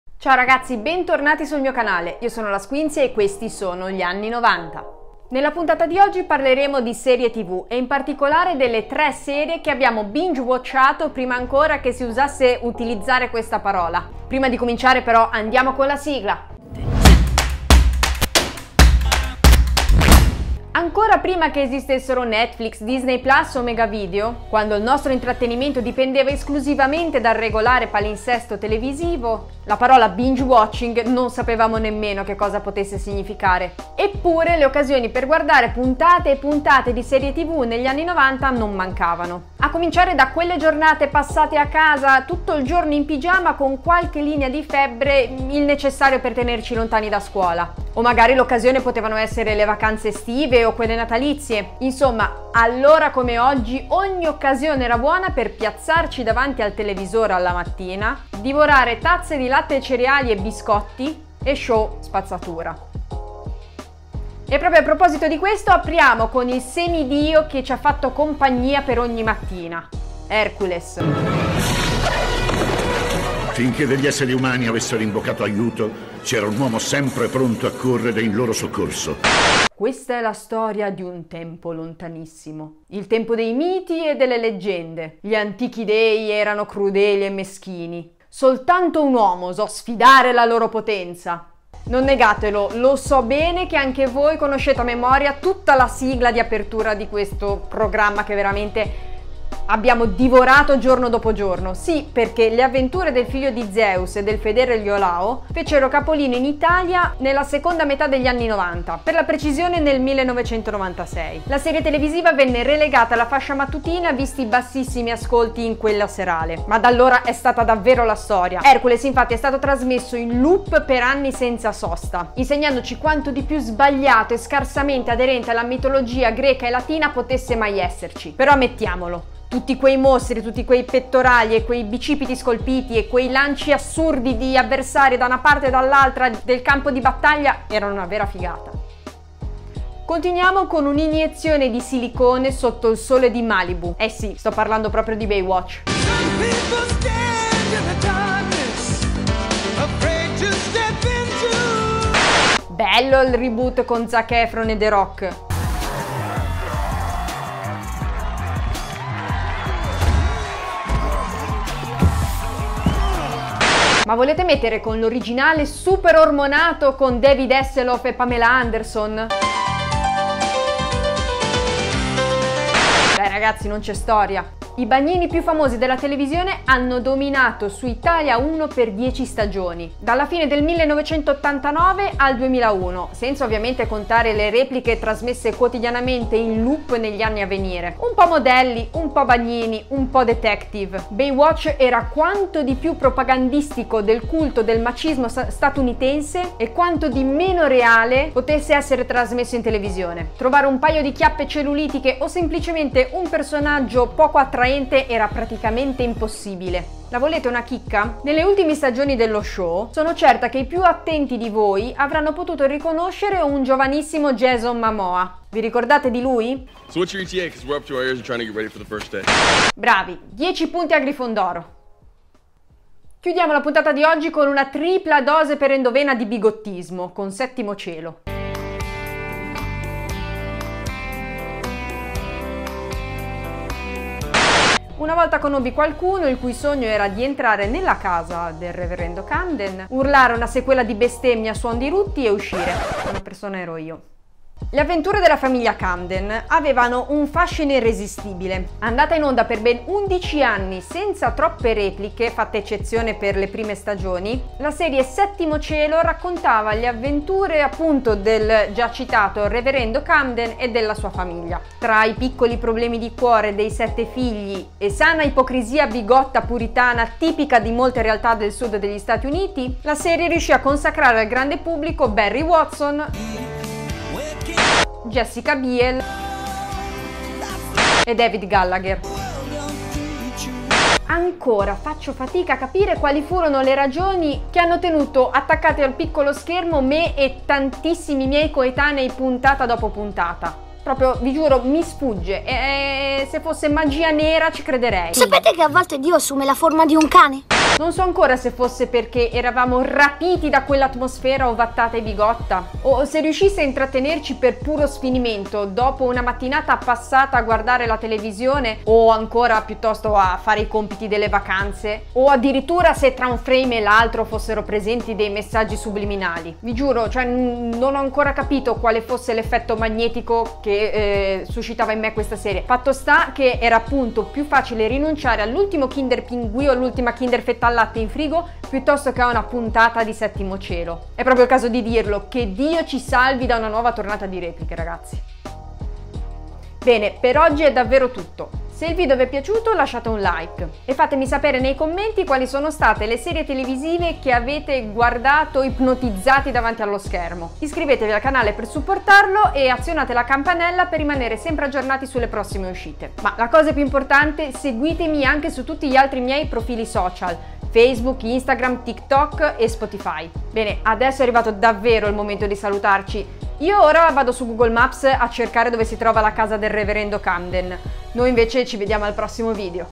MIGLIORI SERIE TV ANNI '90 | LE SERIE TV CULT CHE ABBIAMO VISTO FINO ALLO SFINIMENTO!
Negli anni '90 divoravamo serie tv a profusione, anche senza Netflix. La vecchia e cara tv generalista ci offriva comunque l'opportunità di ammazzarci di serie TV. E alcune le guardavamo senza nemmeno sapere il perché!
Quali sono le tue serie tv preferite degli anni ‘90? Fammelo sapere con un commento!